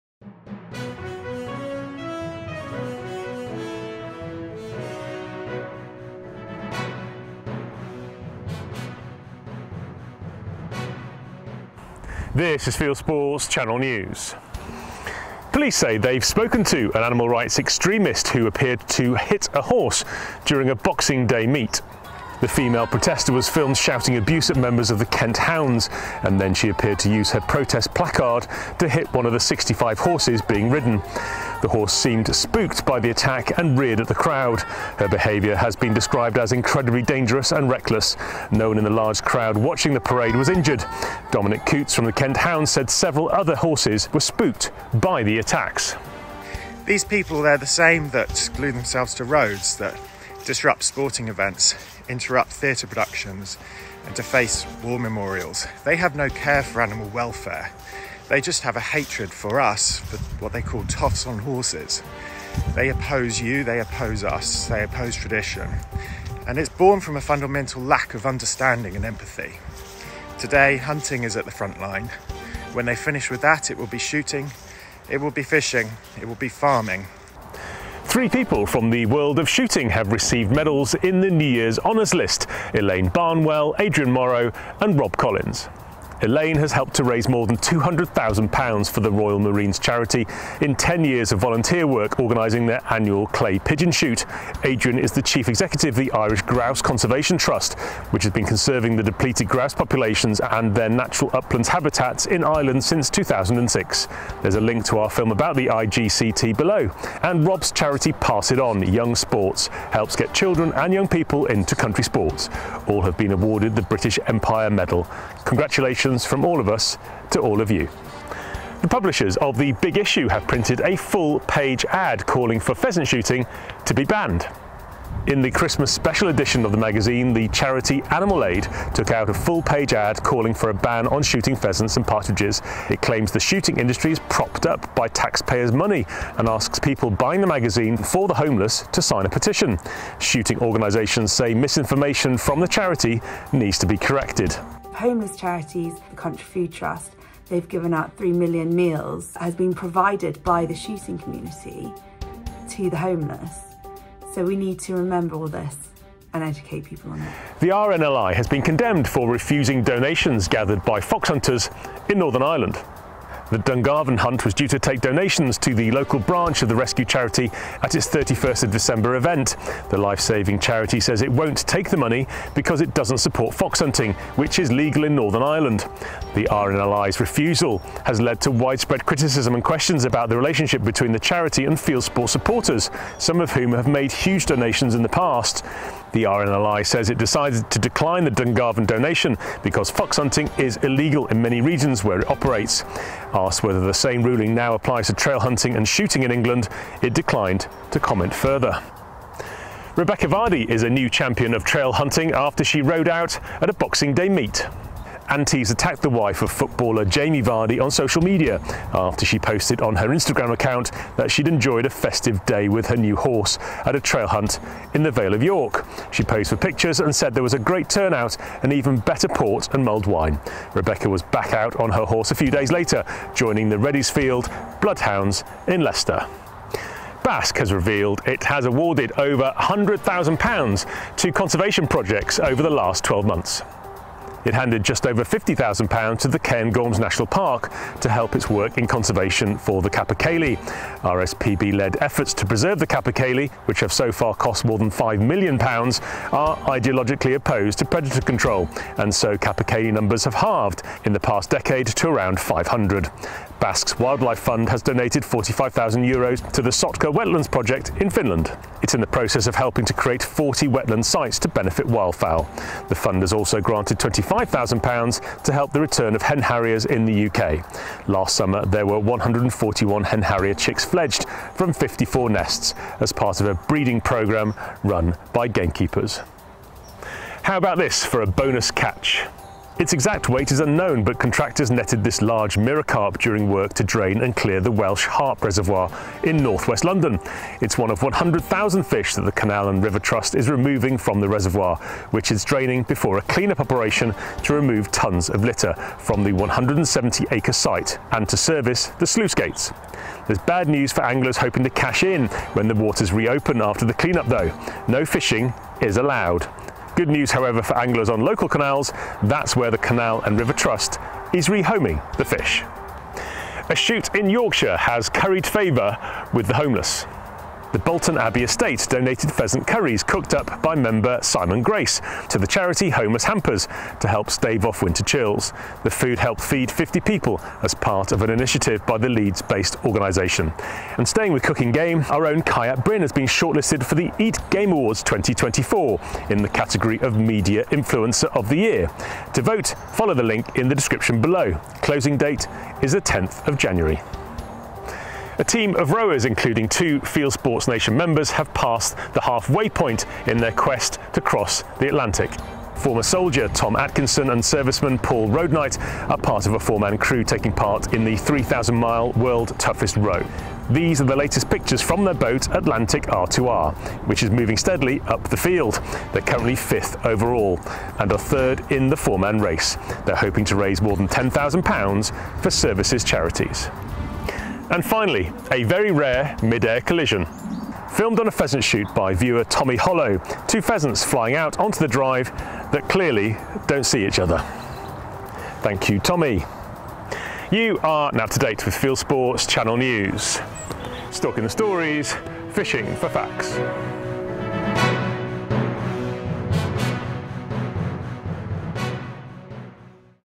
Anti spooks horse at Boxing Day meet – Fieldsports News, 3 January 2024
Shooters in New Year Honours list RM Charity Irish Game Conservation and Pass it on Young Sports

▶ Shoot responsibly
Take part in nature.